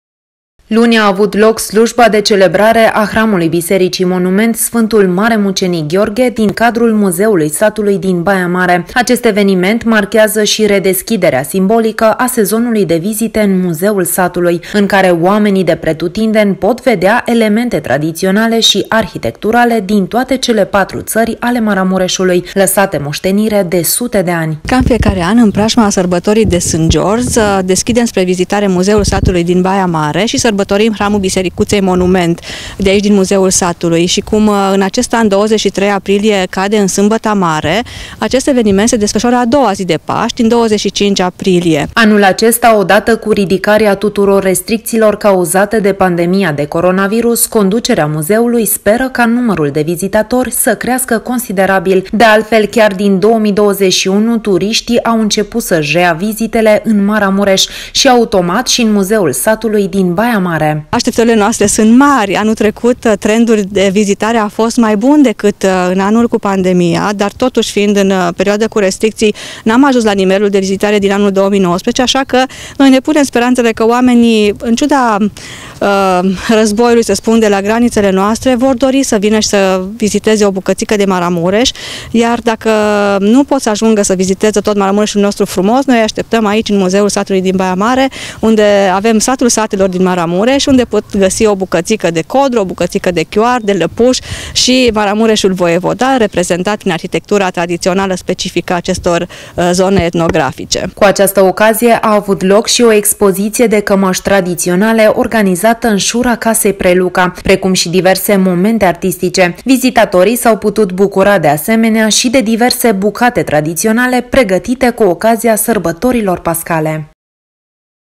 MUZEUL SATULUI S-A DESCHIS SIMBOLIC, CHIAR DE PAȘTE
Ca în fiecare an, slujba de celebrare a Hramului Bisericii Monument “Sfântul Mare Mucenic Gheorghe”, din cadrul Muzeului Satului din Baia Mare, marchează și redeschiderea simbolică a sezonului de vizite. Anul acesta, manifestarea a avut loc chiar în prima zi de Paște.